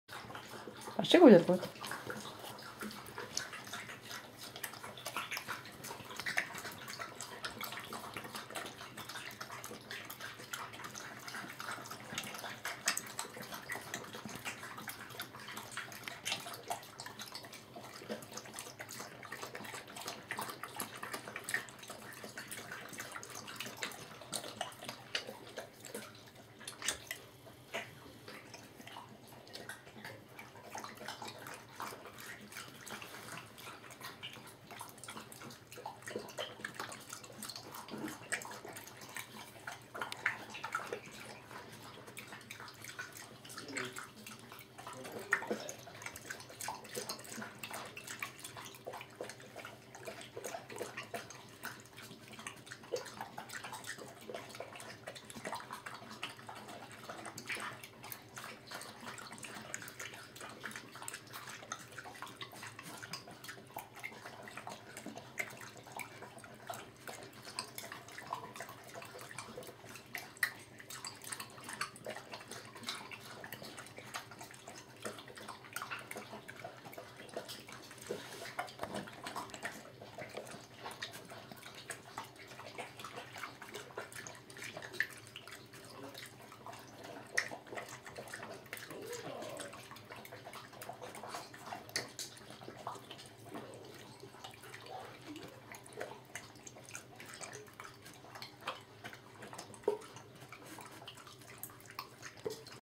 110315algotk9nr1.MOD
Emenems Algot serveras K9 Natural för första gången!!!